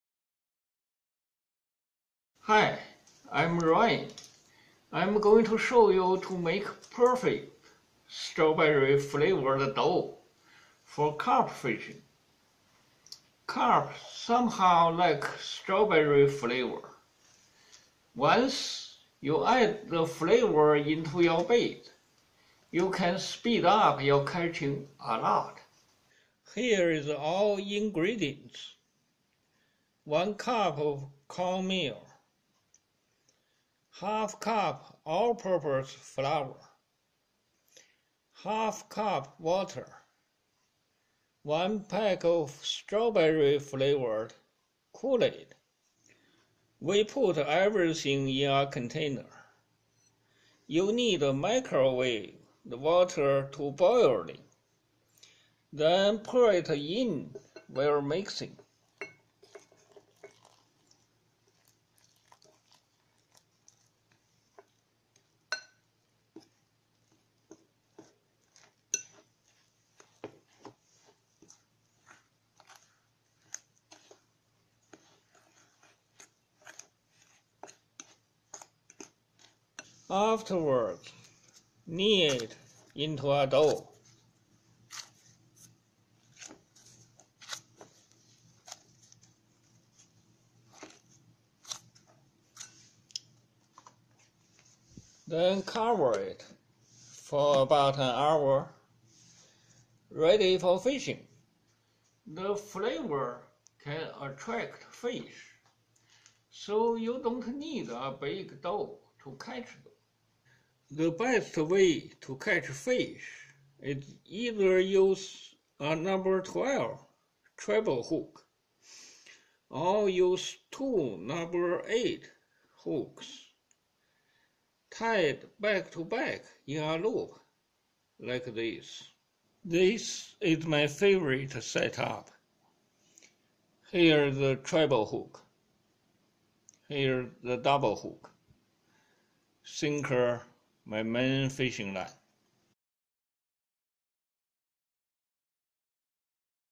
Carp fishing with strawberry dough
Carp somehow likes strawberry flavor. Once you add the flavor into your bait, you can speed up your catching a lot! This strawberry flavored dough also can catch rainbow trout and catfish too.